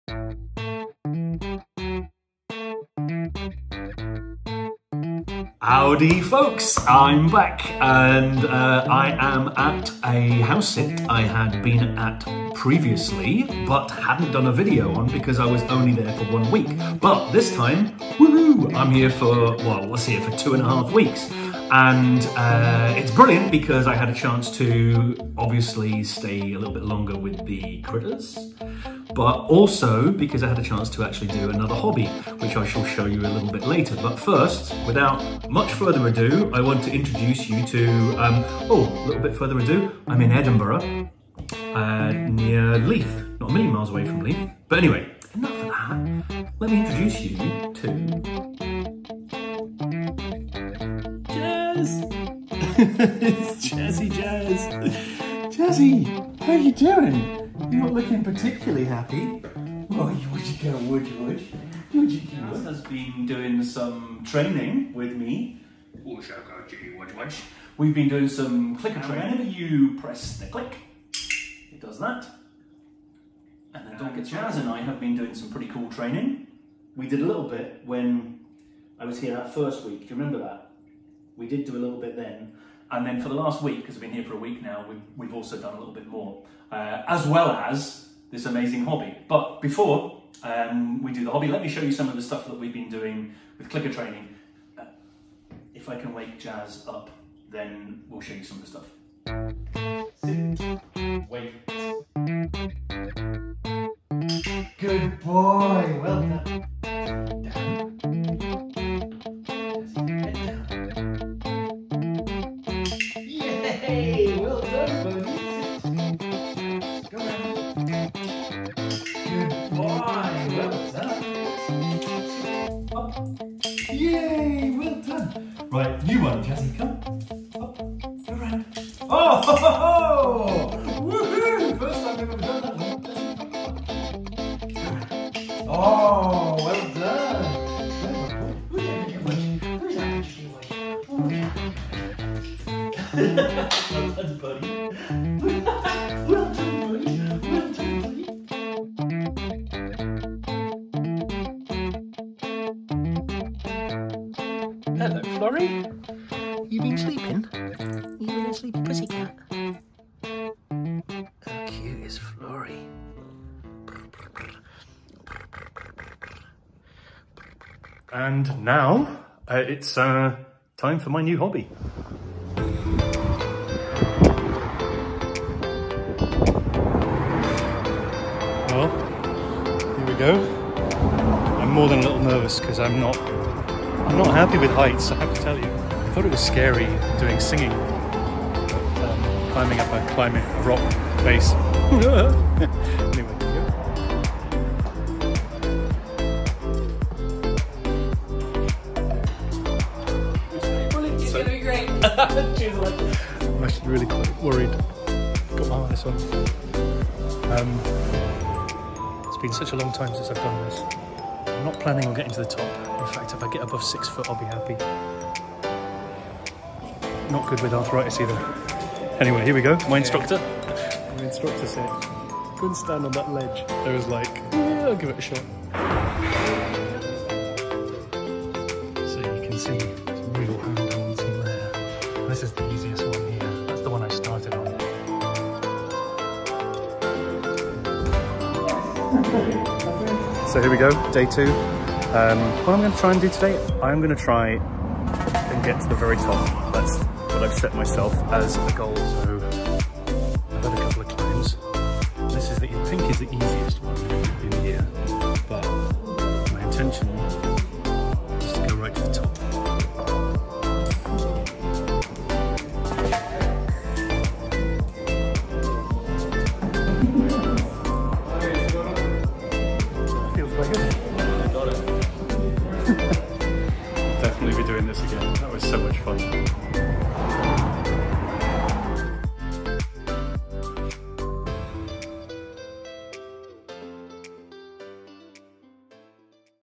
Jazz & Florrie in Edinburgh - July 2023
An awesome return to Edinburgh to help out a family for the second time. Over the moon to have a chance to look after these two critters again, to see my Edinburgh chums and to try out a new hobby. Seriously, get into the habit of trying new things. When you get a bit older you know you arent going to become world champion or even compete.. This takes all the pressure off you to be great, or even good.. It’s a fab time to do new stuff.